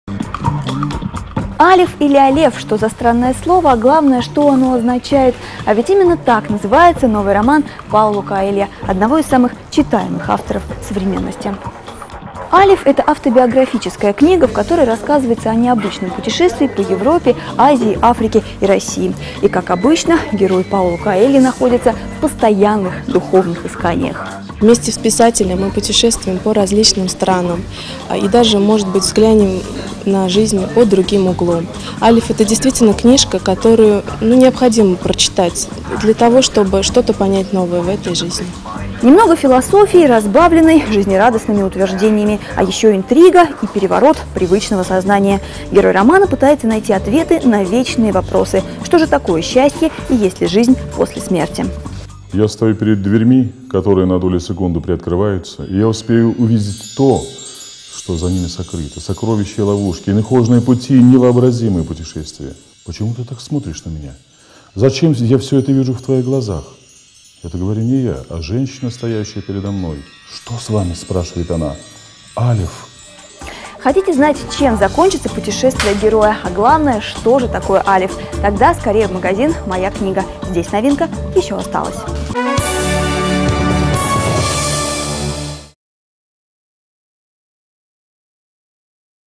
Еженедельная рубрика "Книжная полка" АЛЕФ
Когда-то наша страна была самой читающей в мире. Сейчас интернет и телевидение с успехом заменяю печатные книги. Это несправедливо посчитали мы. И с сегодняшнего дня запускаем новую рубрику - Книжная полка. Раз в неделю мы с помощью известных саратовцев будем рассказывать о главных новинках издательского бизнеса...